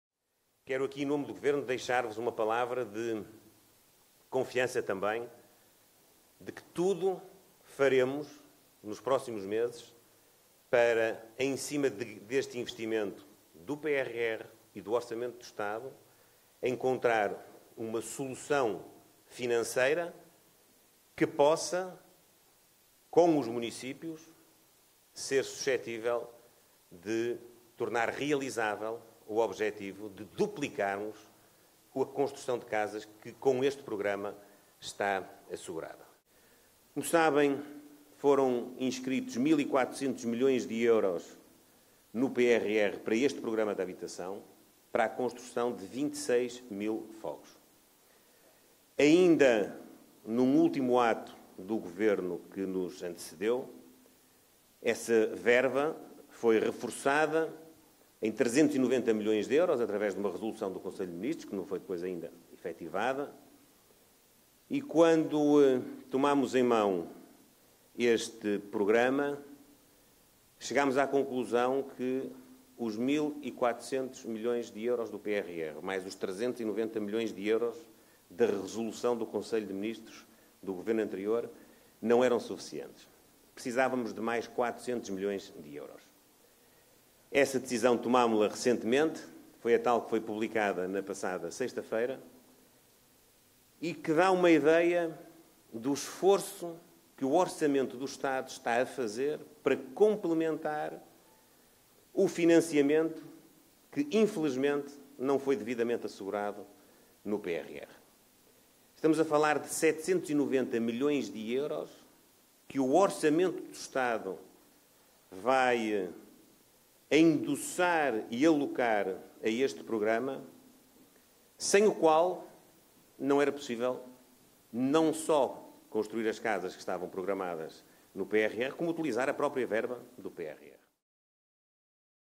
Montenegro promete “tudo fazer” para tentar construir dobro das casas previsto no PRR
O primeiro-ministro, Luís Montenegro, prometeu que o Governo “tudo fará” para encontrar uma “solução financeira” que permita, em conjunto com os municípios, construir o dobro das casas previstas no Plano de Recuperação e Resiliência, que é de 26 mil fogos.